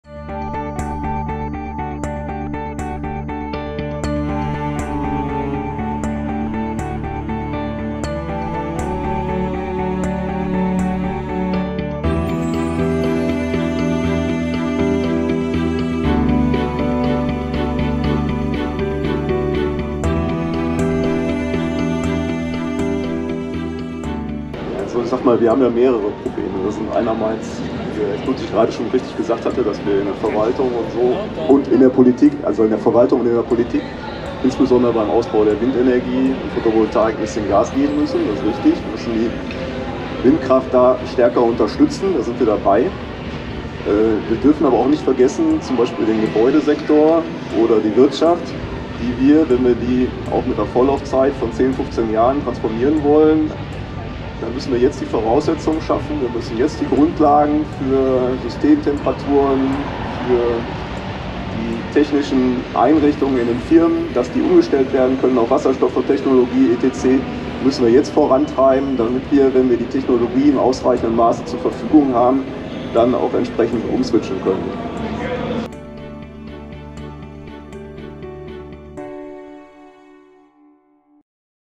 Unser Ratsherr und umweltpolitischer Sprecher Jörg Bogoczek bei der Klimawerkstatt in Aachen
Bei der Klimawerkstatt am 22.09 fand eine Podiumsdiskussion mit dem Thema: "Wie kann eine Energiewende in Aachen funktionieren?"